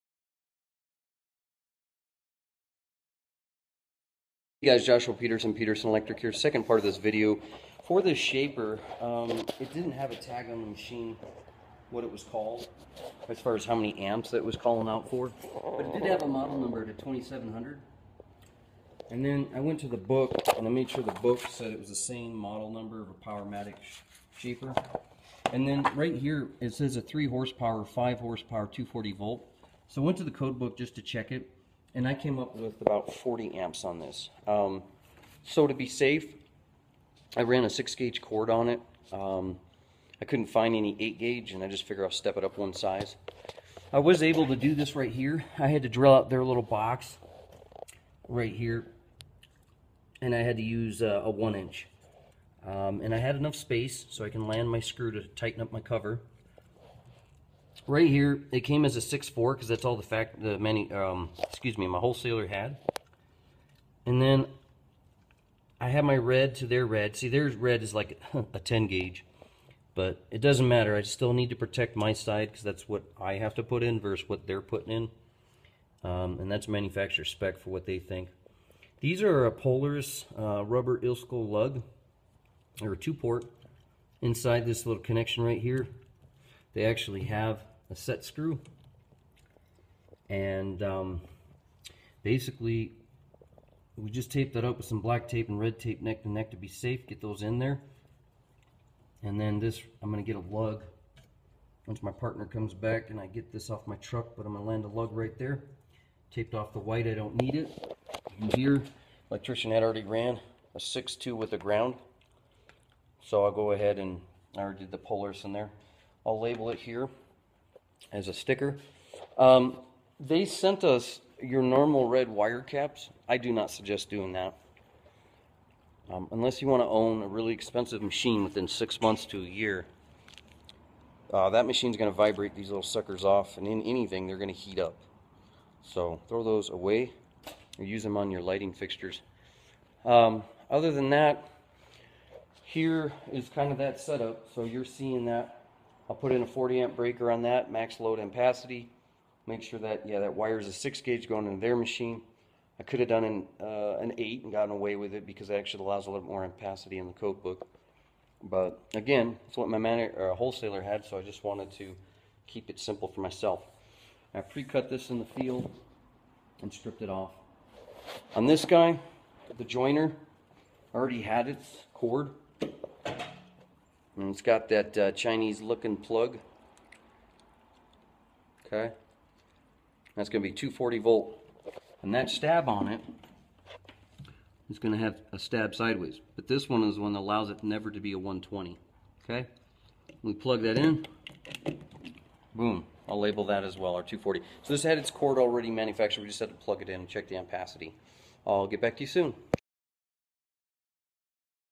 Determining Wiring Needed For Powermatic Wood Shaper Machine
- Part 2 Talking about the wiring needed for Powermatic equipment in a wood shop.